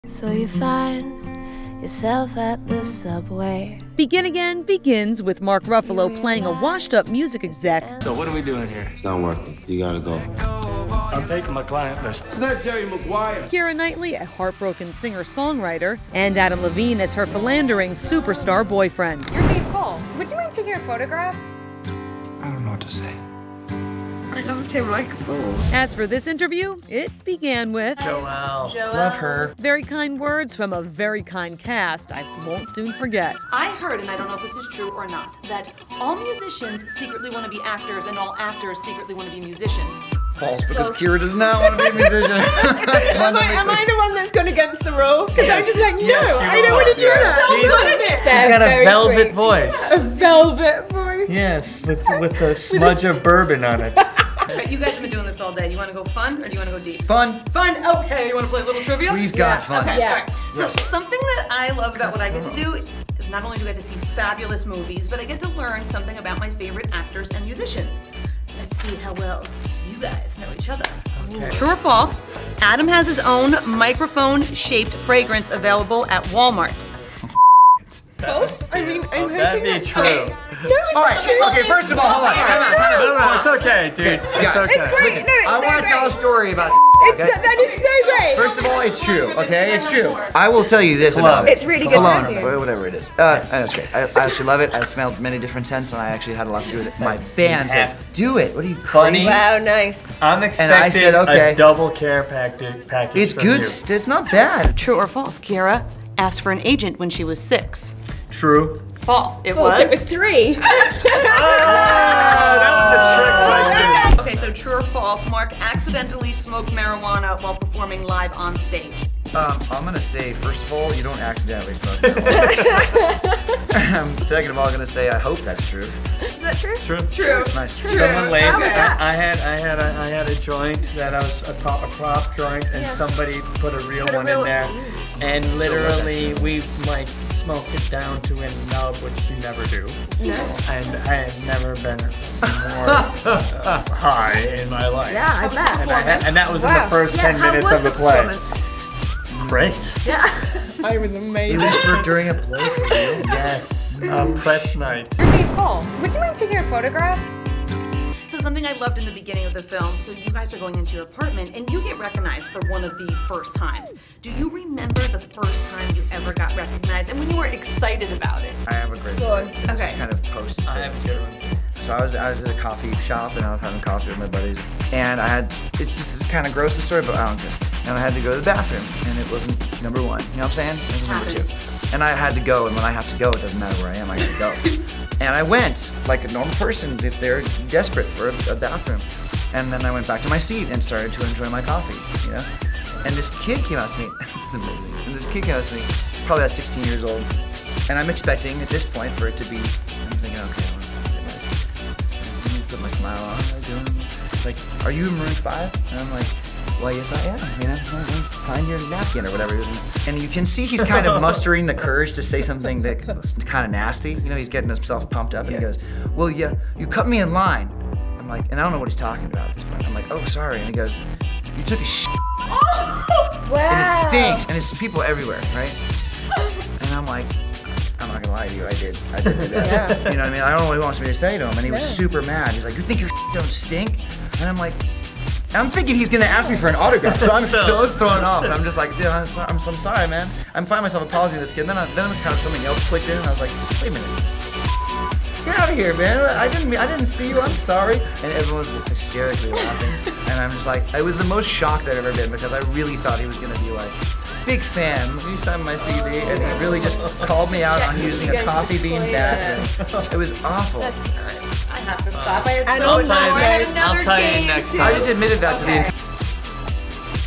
Adam Levine, Keira Knightley & Mark Ruffalo Unfiltered BEGIN AGAIN Interviews
WATCH: BEGIN AGAIN Cast Unfiltered like you've never seen before.
Adam Levine, Keira Knightley and Mark Ruffalo dish on being recognized for the first time and play a little game of getting to know one another.